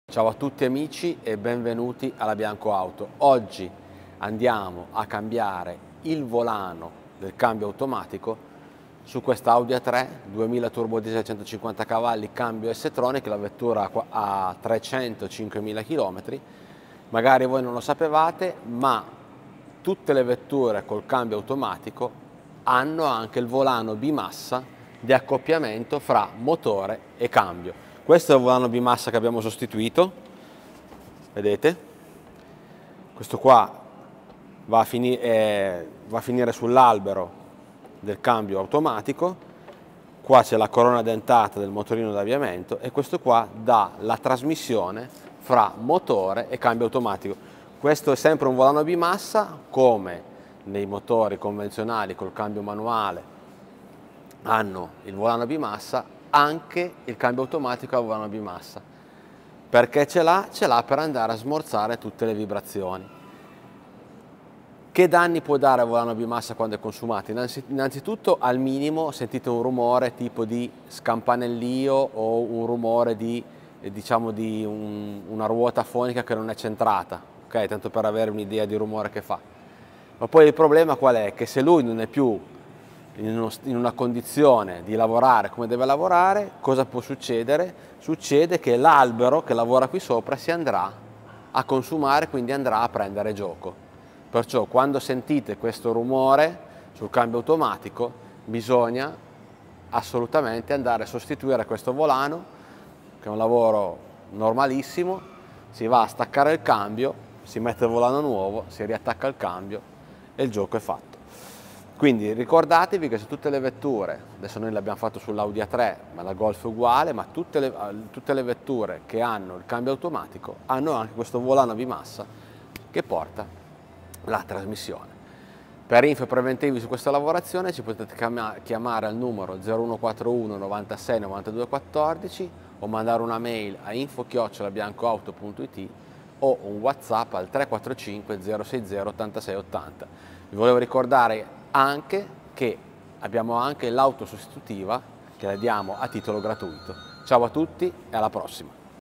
Sostituzione volano bimassa su Audi A3 sportback 2.0 tdi con cambio automatico s-tronic
Auto sostitutiva gratuita